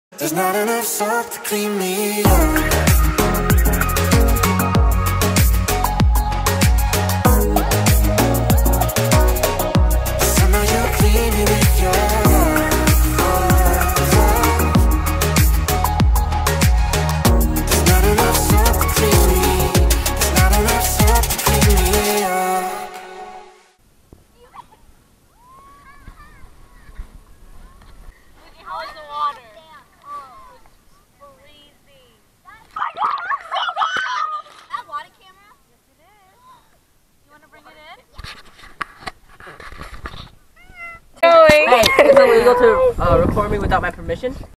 Family Picnic || DOOR COUNTY
A little video from our family picnic up in door county! We ate lots of food, swam in 50 degree water, played games, danced and just hung around.